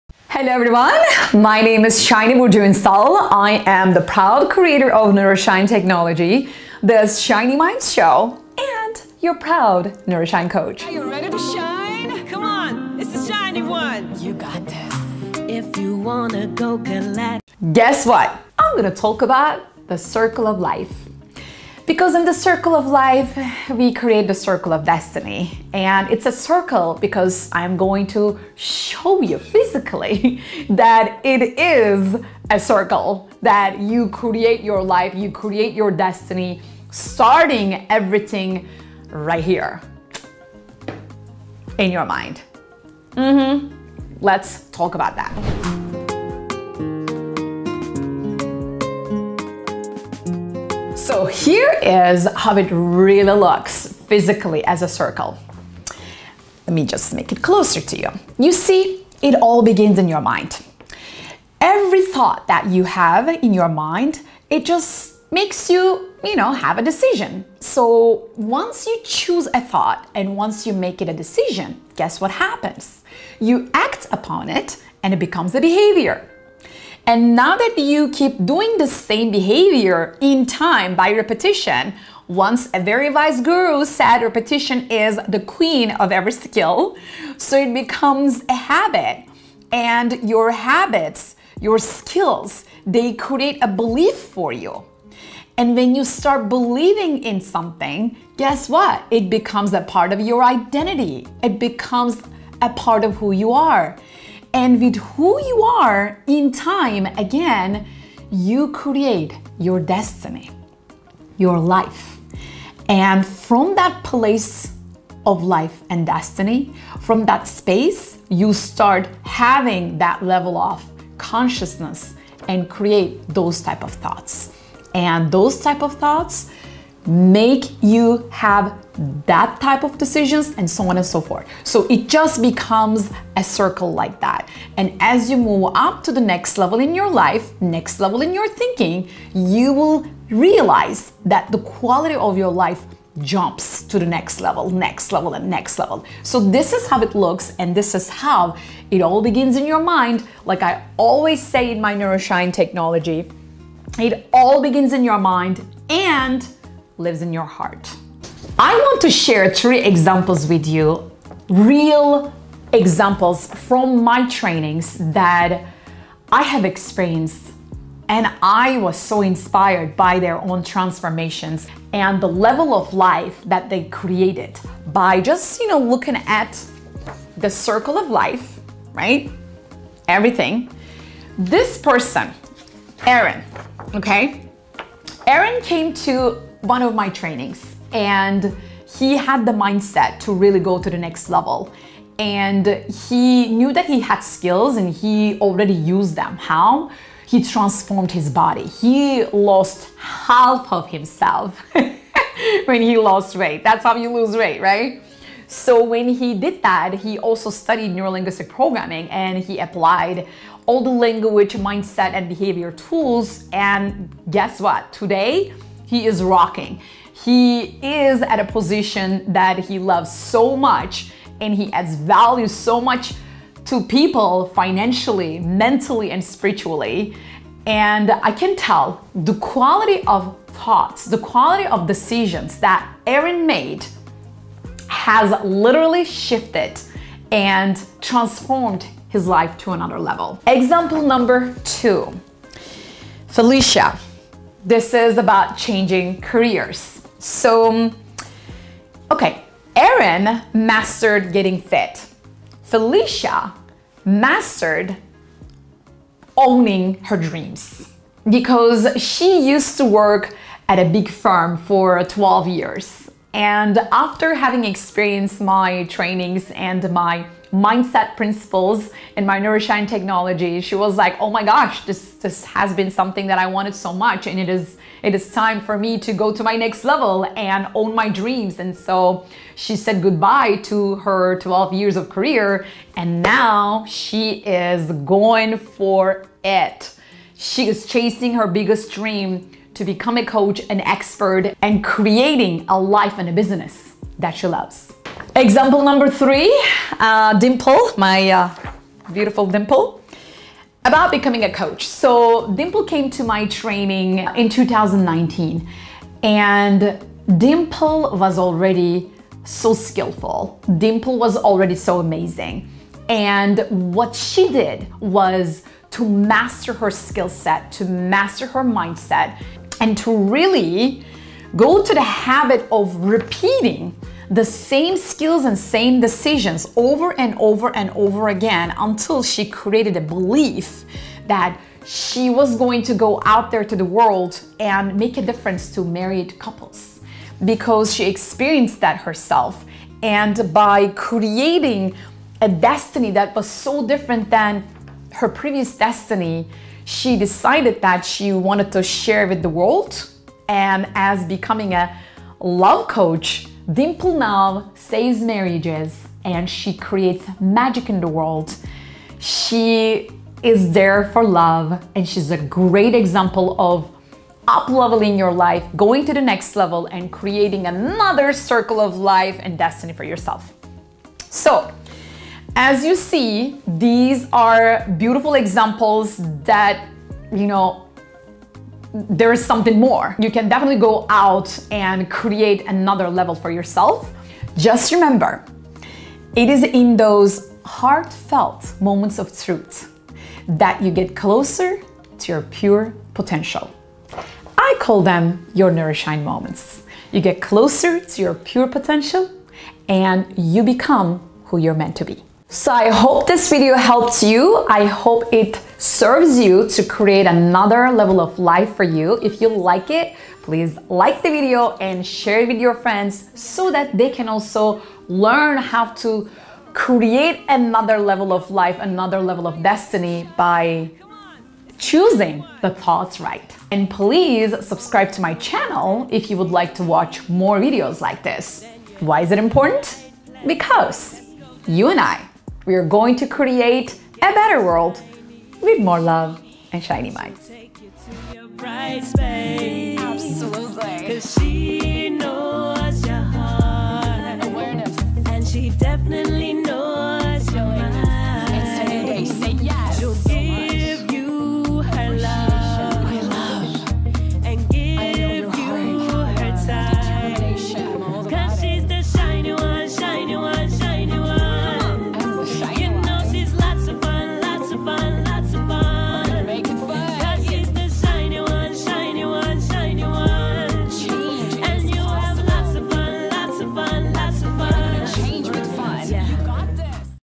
🌠 Beyond Goals & Dreams: Discover Your Magnetic 🧲 Circle of Destiny
Do you feel like you're stuck? Want to live a life full of passion, meaning, and happiness? The Circle of Destiny is your key to unlocking that dream life! ✨

This video will show you a powerful concept that explains how your thoughts, choices, habits, and beliefs shape your future.

Here's how:
✔️ Positive thinking requires abandoning "I can't" statements in favor of "I can" and "I will" ones.
✔️ Do good things: Swap bad habits for good ones.
✔️ Listen to your heart: Make choices that feel right and lead you closer to your dreams.
✔️ Live with passion: Fill your days with things you love and light your soul on fire!

No more feeling lost or bored.
It's time to take charge of your life and make it amazing!

Remember, you have the power to change your life, one spin at a time! ✨

Join me on this journey to discover your own magical Circle of Destiny!

Let's go! 🚀

Made with love ❤️ for a better world, with more love ❤️ and ShinyMinds.

by Shiny Burcu Unsal